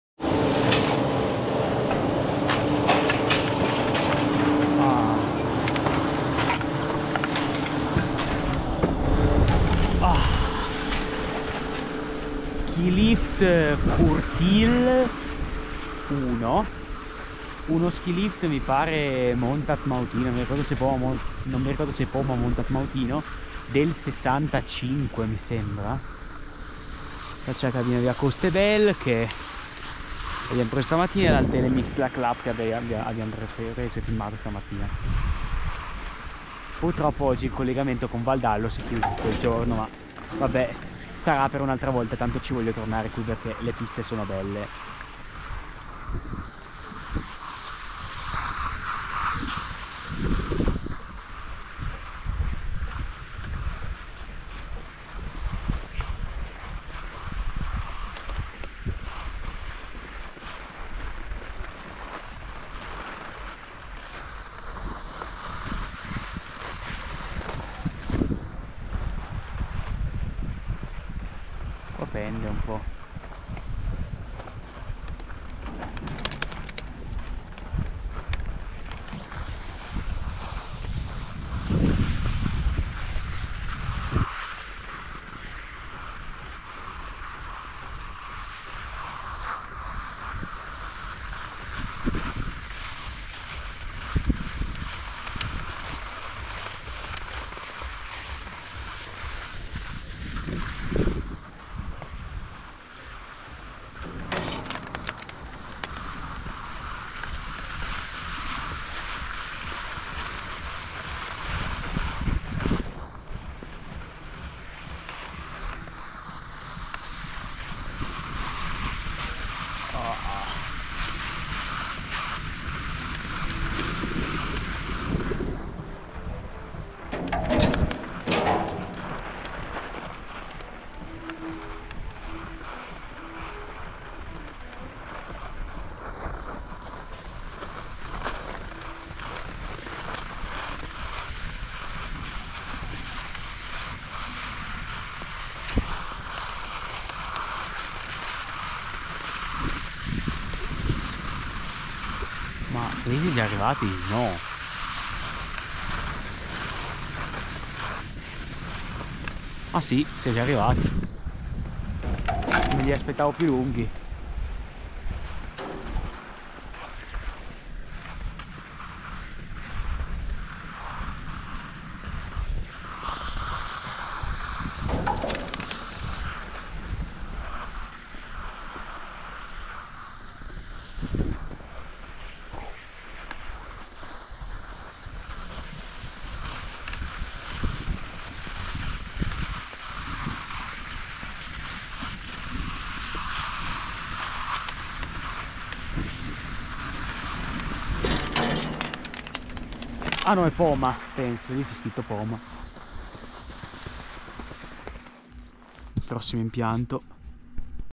Risalita integrale skilift Poma "Courtil 1" Pra Loup (Francia)
Skilift Poma del 1965 affiancato successivamente da uno skilift del medesimo costruttore del 1970.